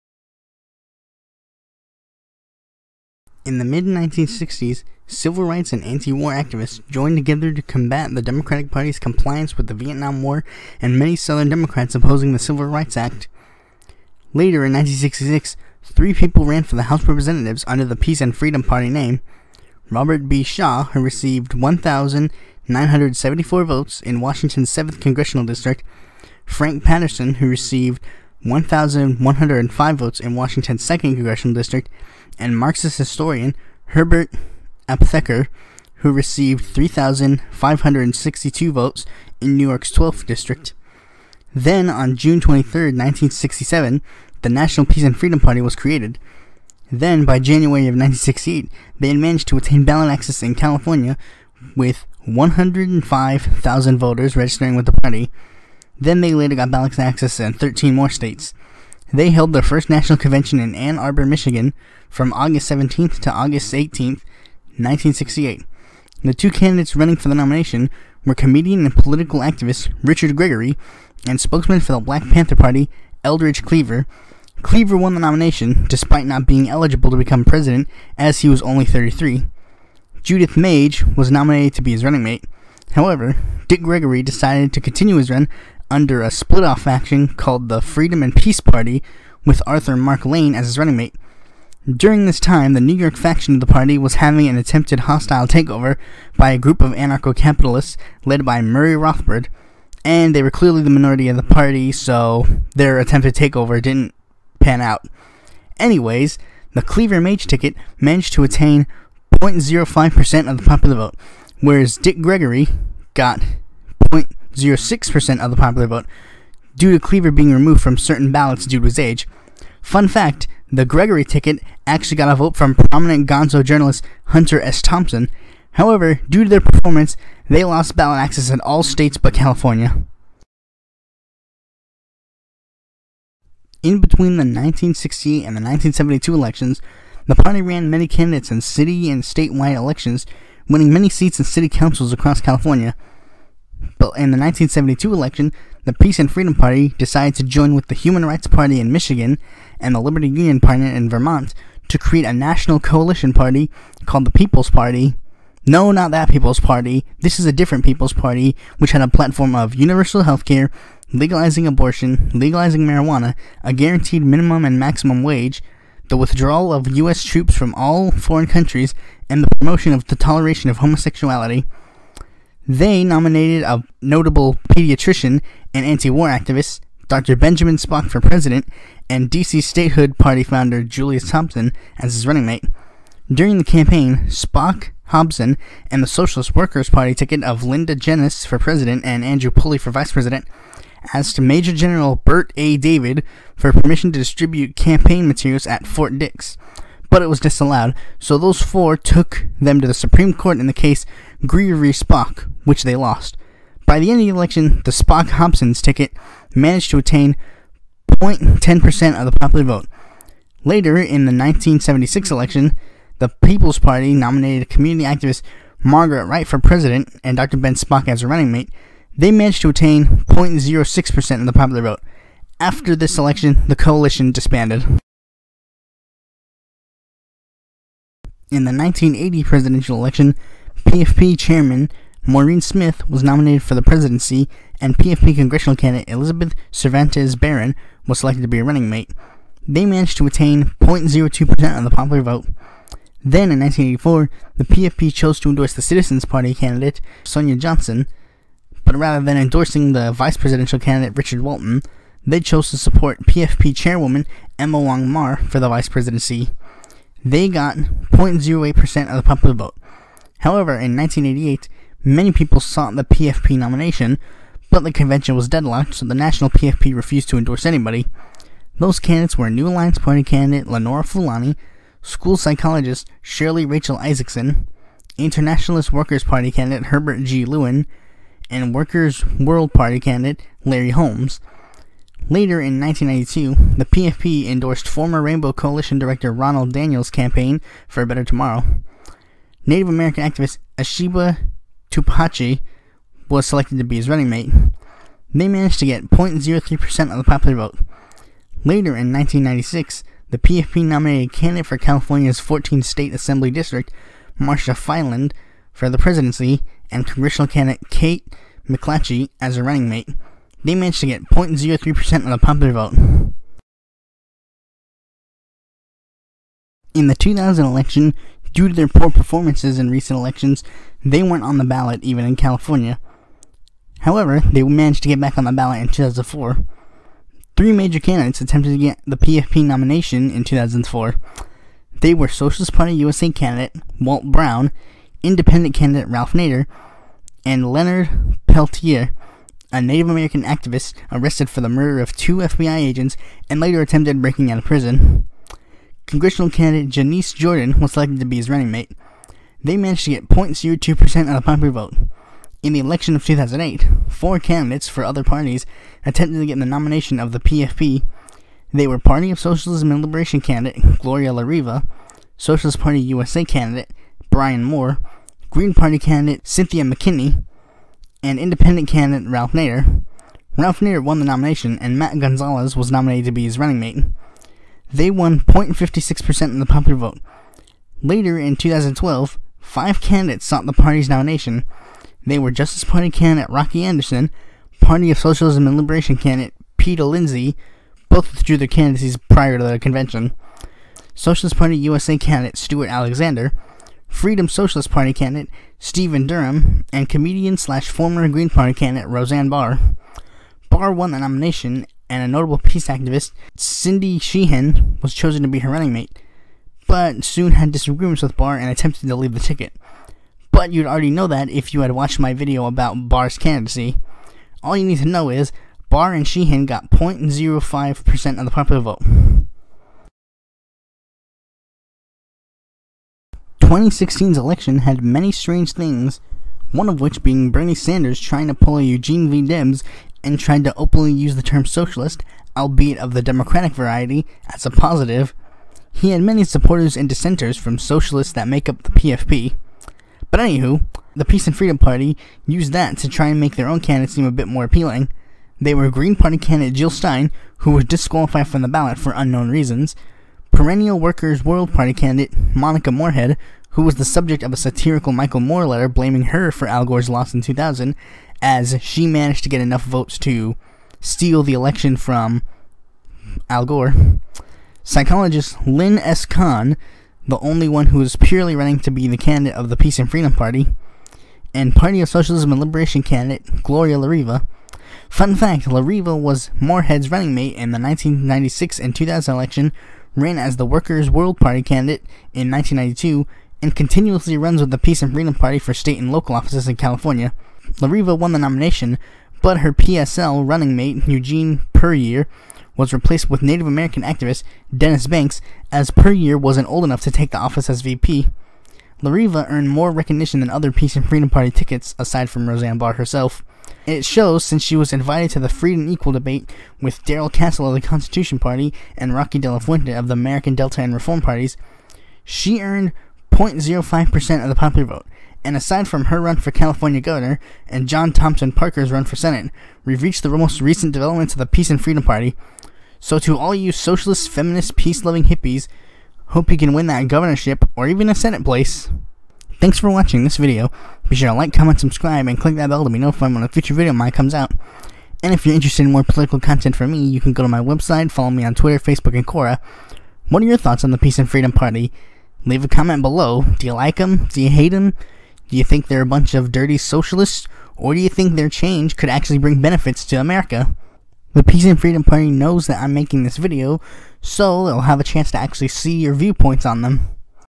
History of the Peace and Freedom Party
In this episode we discuss the history of a Prominent Left-Wing Party, The Peace and Freedom Party.

0:00 The Dove Takes Flight
2:10 Mr Spock, More like DR Spock
3:46 Before Y2K
5:14 New Millennium
7:01 PFP, PSL, GLR